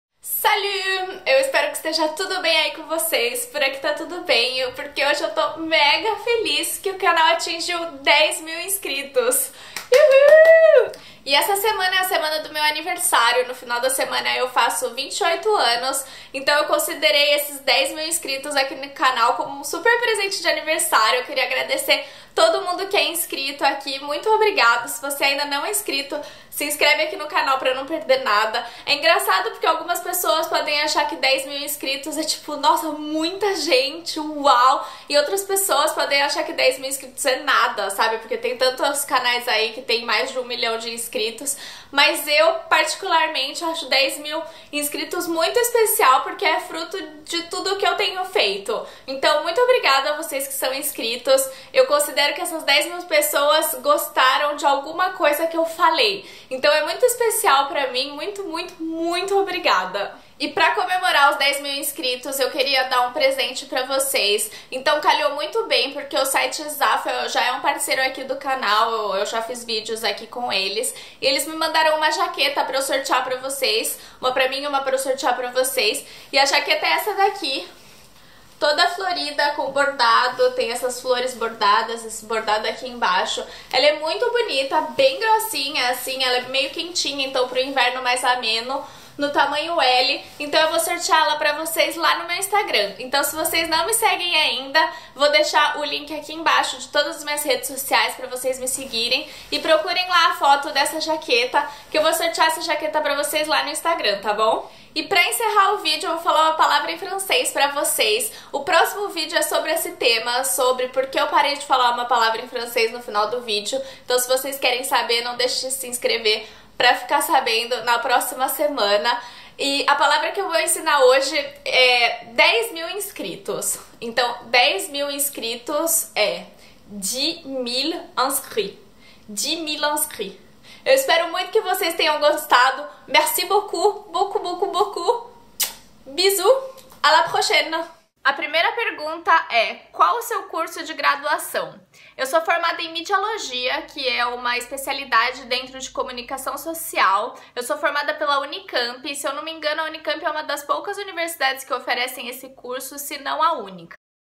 10 Mil inscritos + Sorteio
10 MIL INSCRITOS AQUI NO YOUTUBE!!! Muito feliz e muito agradecida! Obrigada a todo mundo que faz parte desse meu mundo aqui no Youtube. Obrigada pelos comentários, pelo apoio e pelo carinho sempre!

O sorteio é lá no Instagram e é super fácil de participar

♥ Me siga no Snapchat - @flaviapenereiro